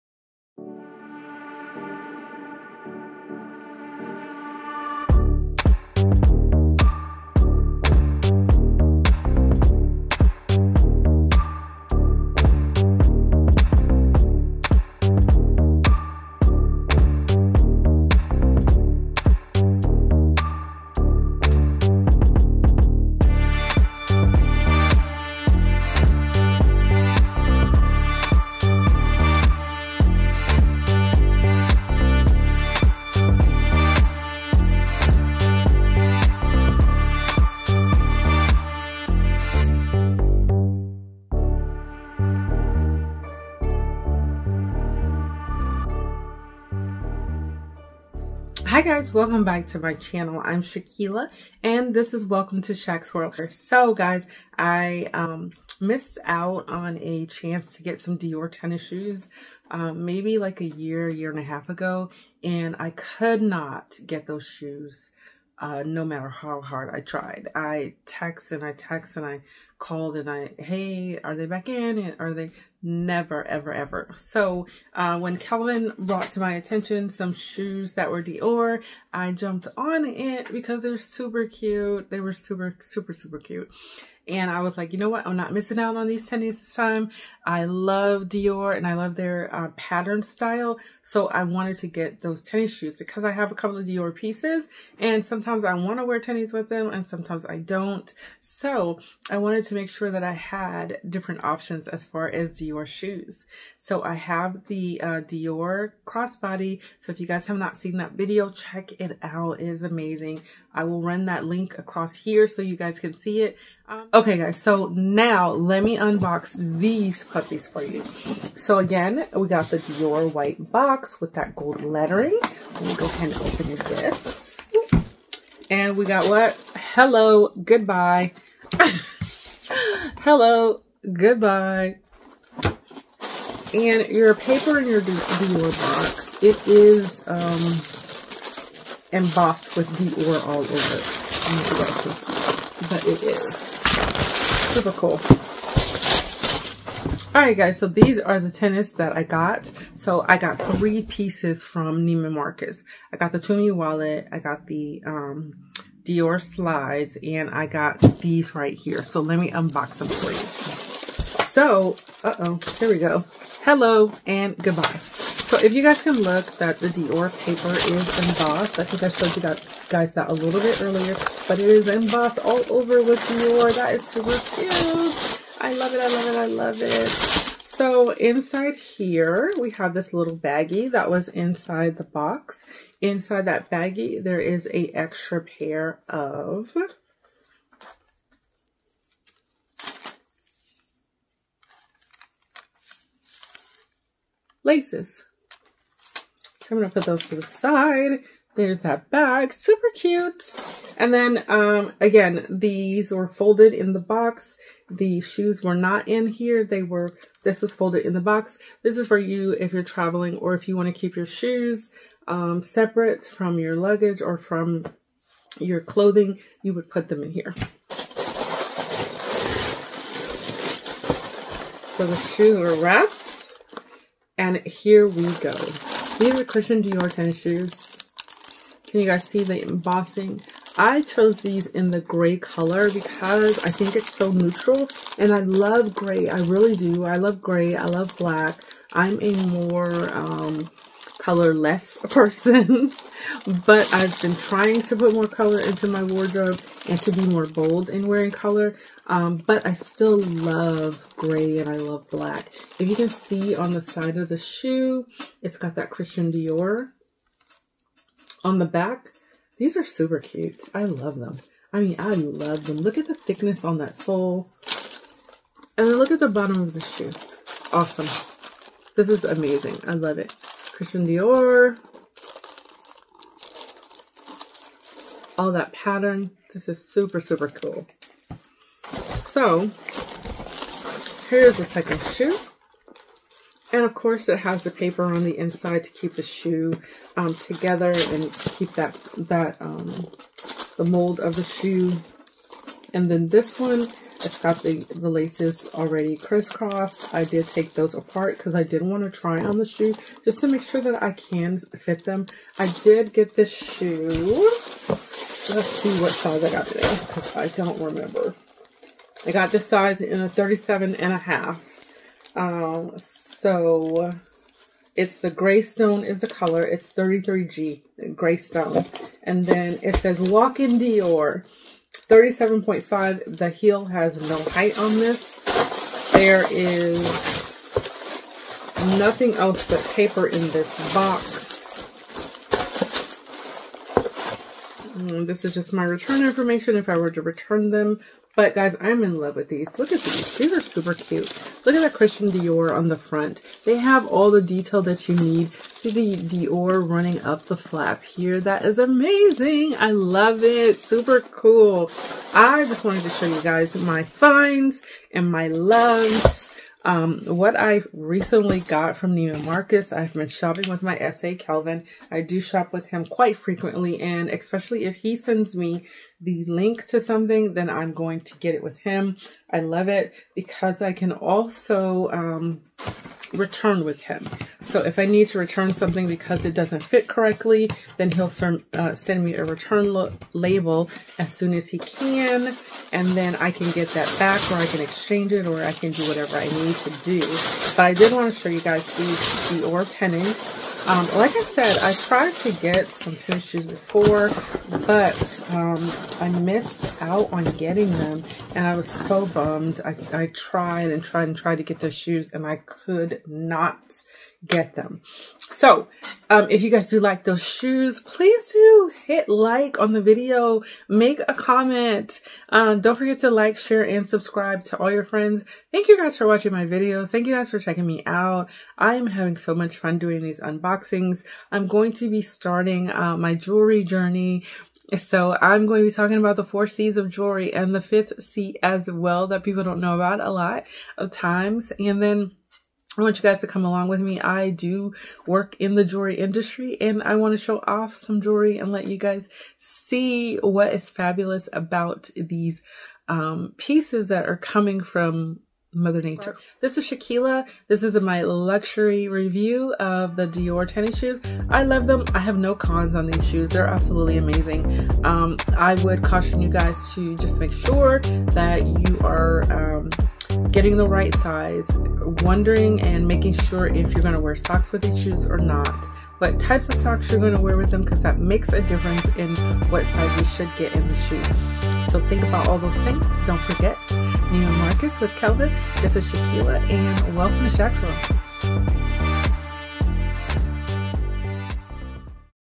DIOR TENNIS
@Dior  tennis unboxing and overview, my review of these designer shoes  @NeimanMarcus